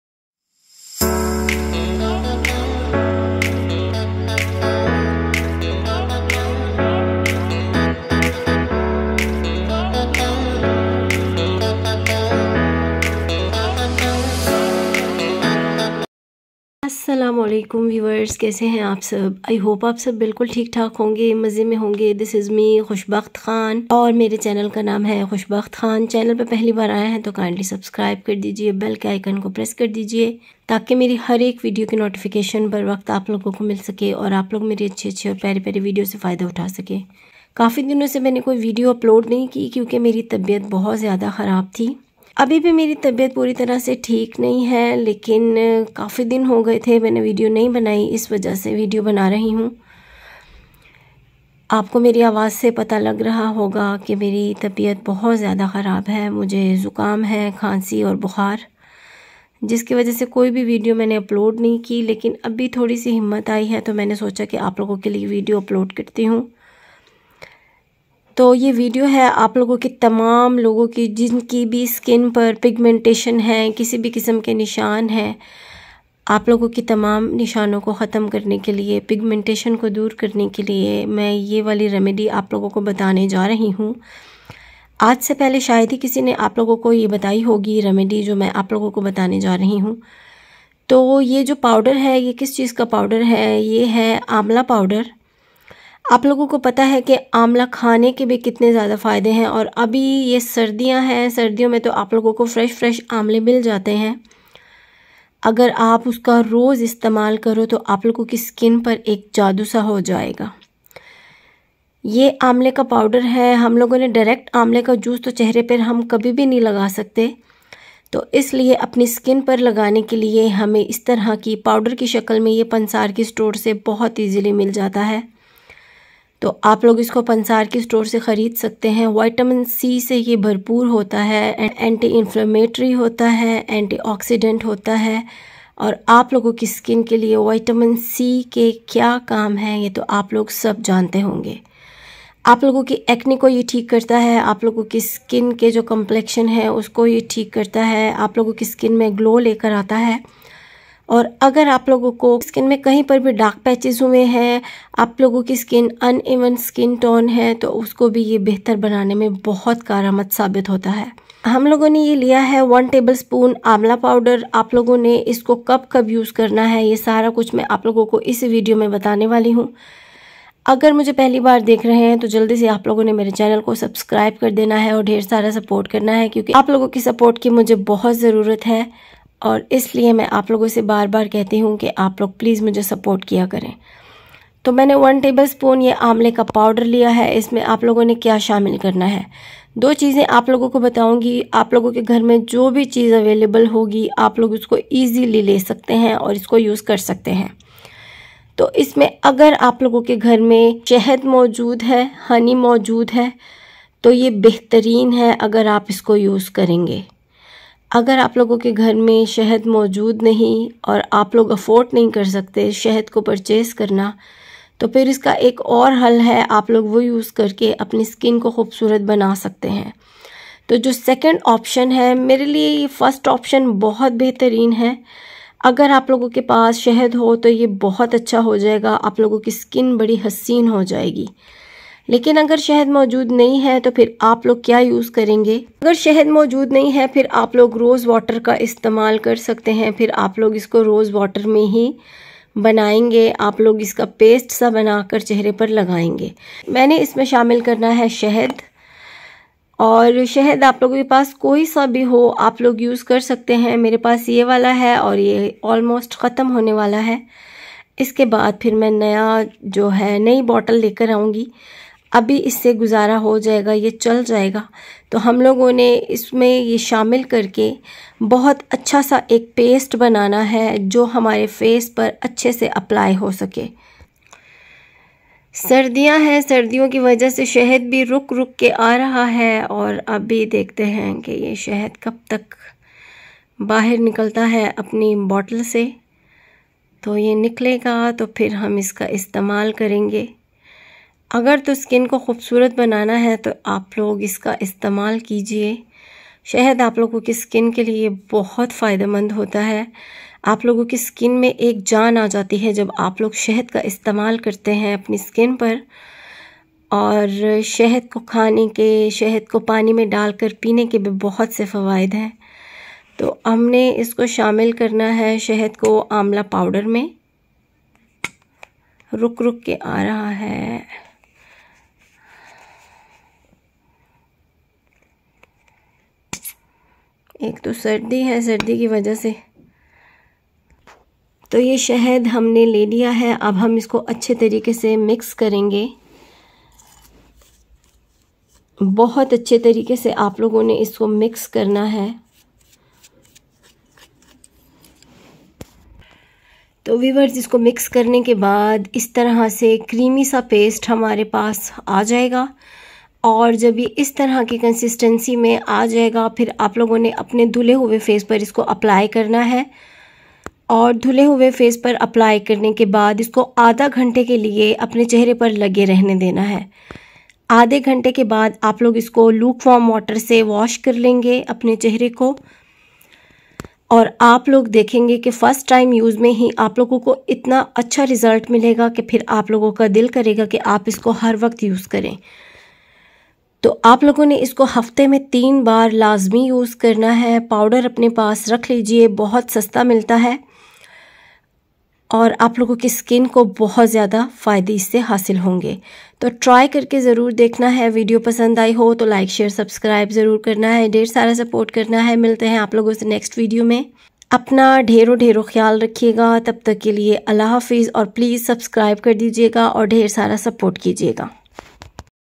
*DIY* Get rid of dark spots, pigmentation, hyperpigmentation and melasma
Remove dark spots, pigmentation, hyperpigmentation and melasma at home with easy home remedy.

thanks
khush bakht khan